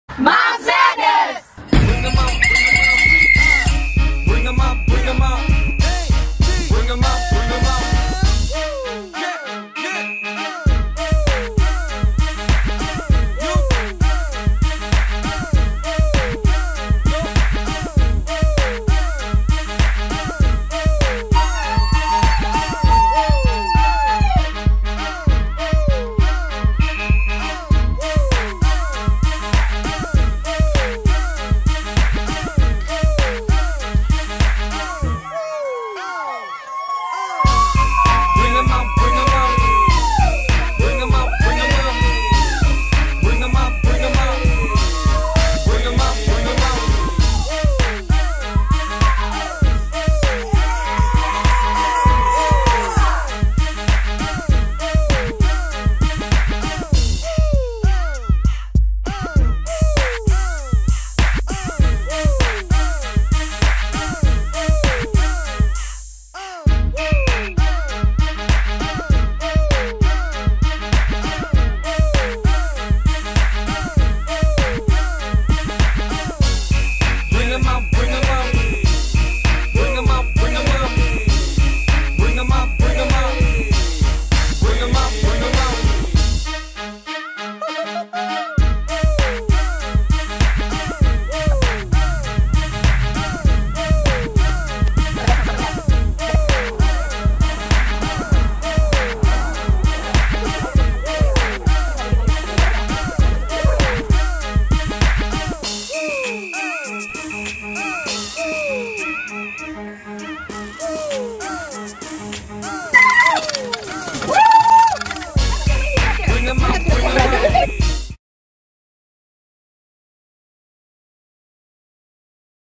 Presenting our MADS Moms!
Have so much fun watching our MADS moms end of the year presentation at our annual banquet